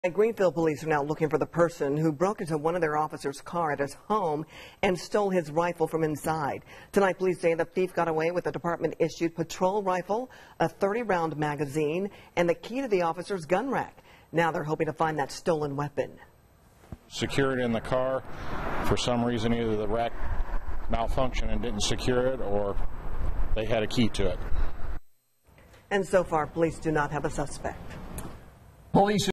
Greenfield cop gun stolen
Greenfield police are now looking for the person who broke into one of their officer's car at his home and stole his rifle. Police say the thief got away. Now they're hoping to find the stolen weapon.